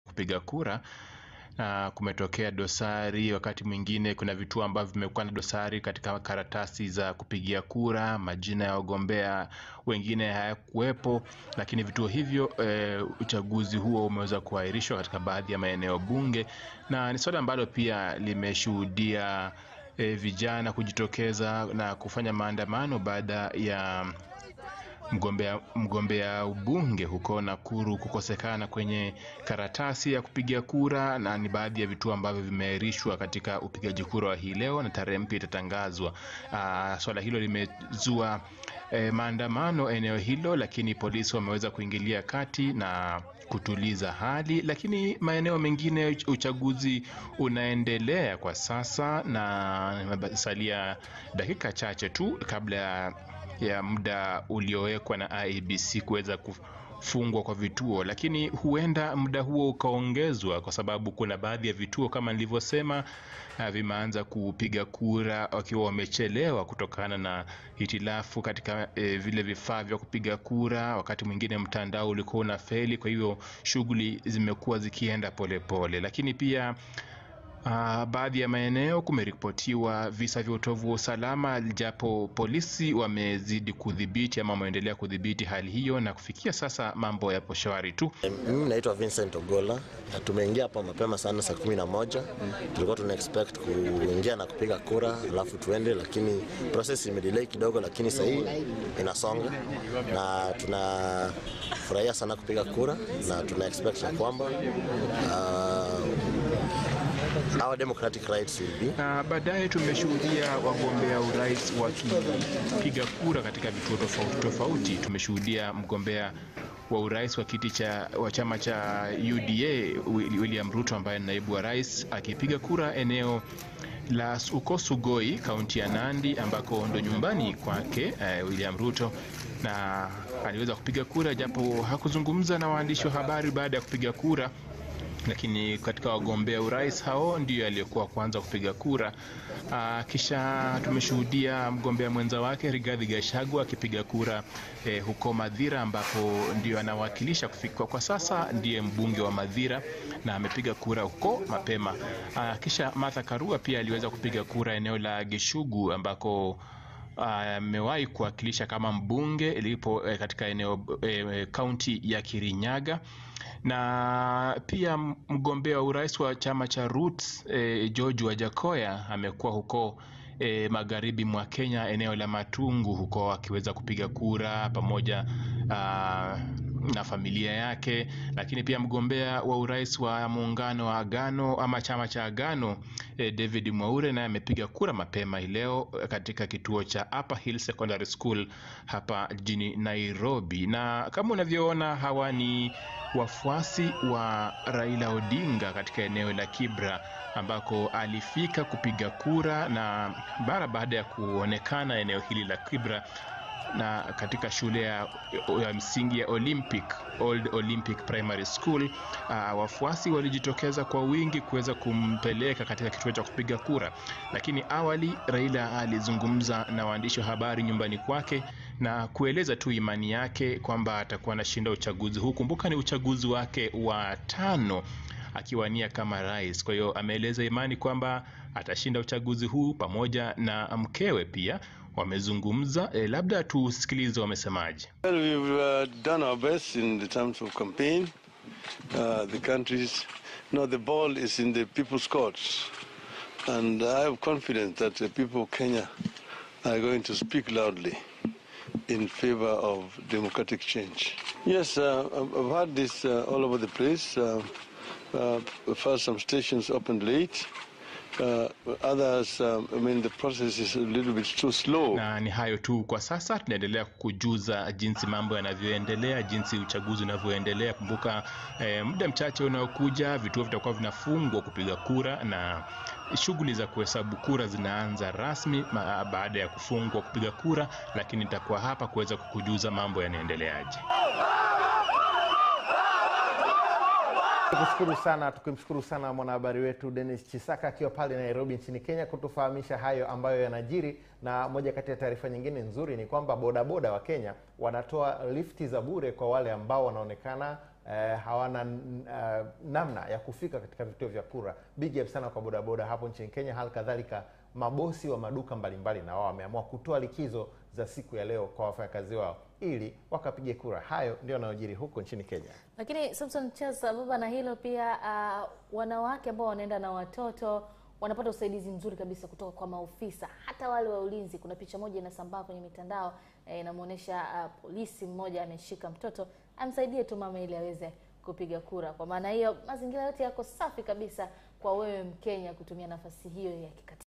DUH!! Nyomi la watu wakimsindiza Raila Odinga kupiga kura nchini Kenya
LIVE: Kinachoendelea Uchaguzi Kenya muda huu | Raila, Ruto ngoma nzito